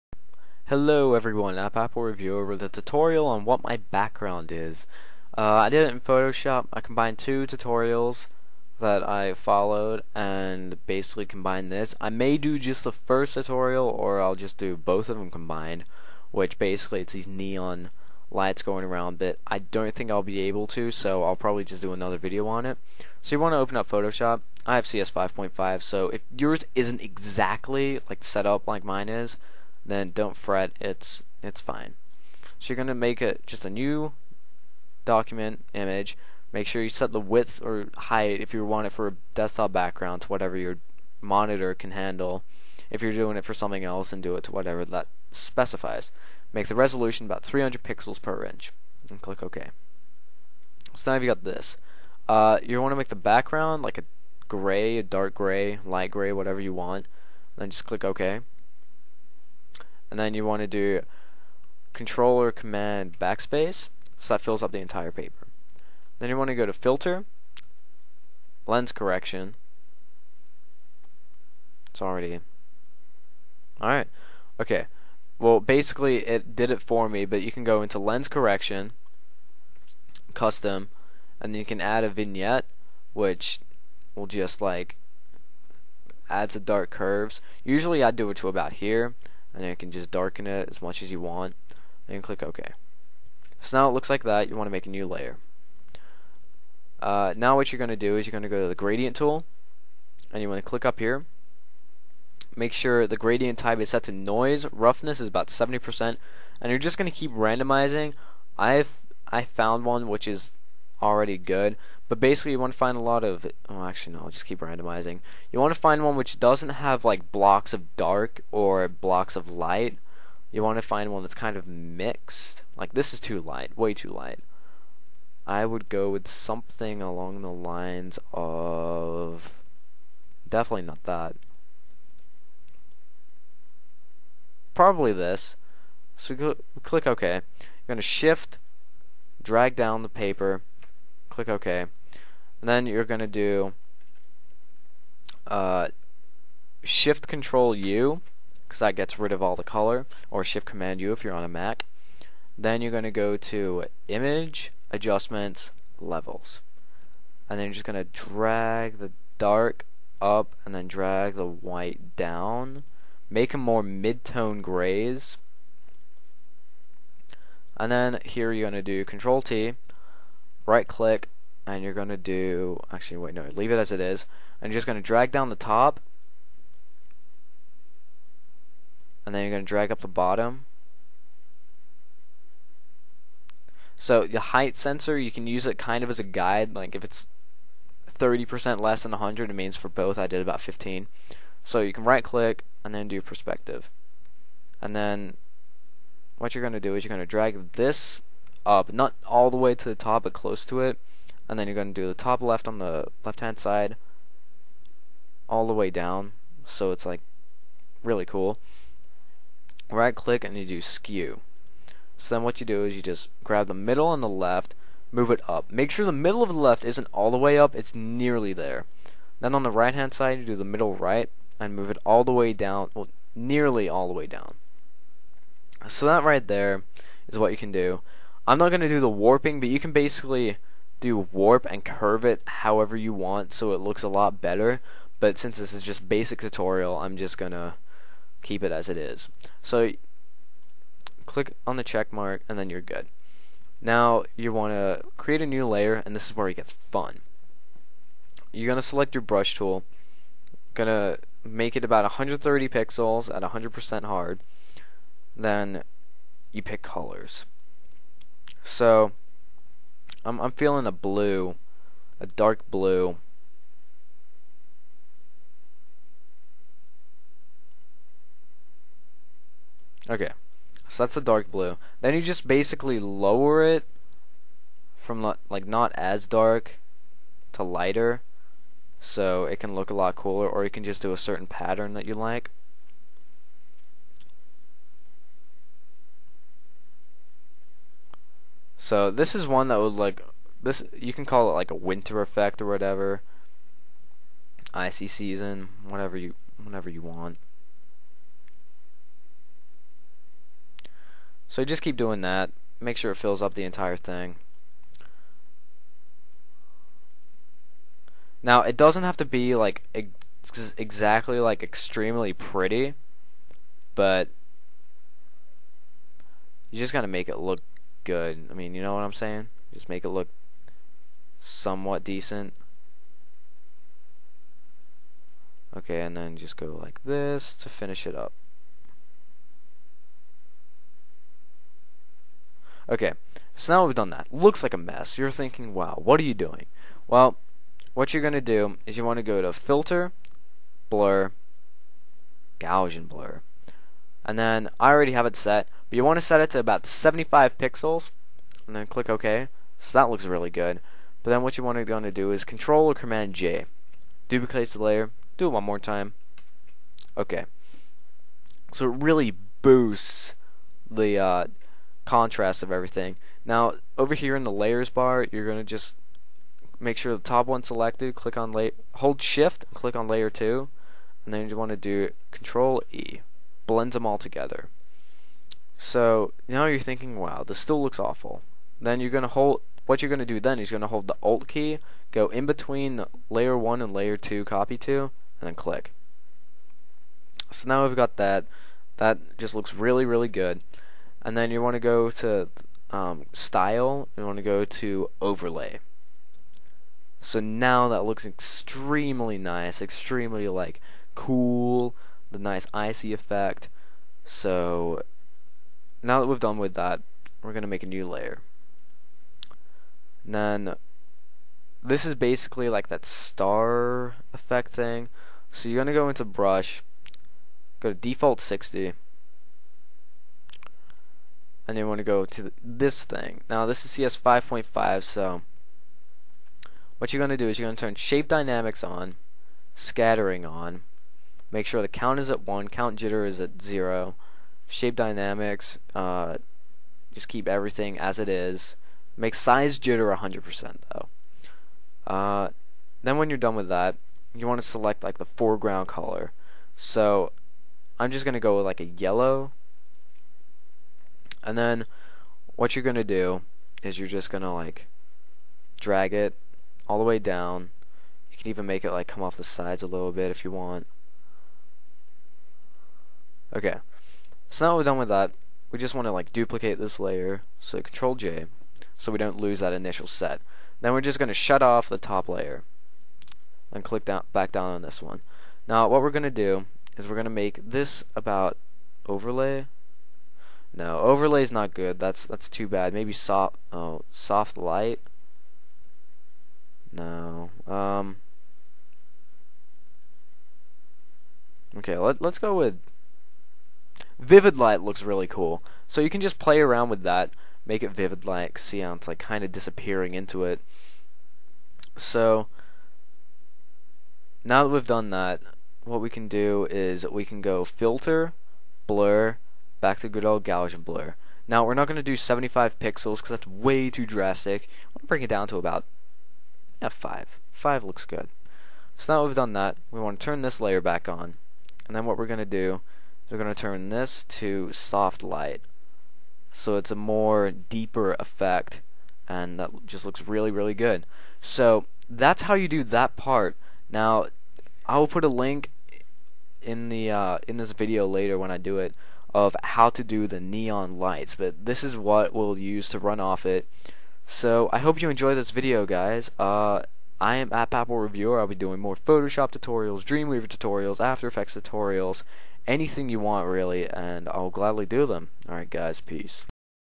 Photoshop Tutorial :: How to make a really cool background!
Quick and relatively simple tutorial for those that want to make a really cool desktop background for themselves or friends!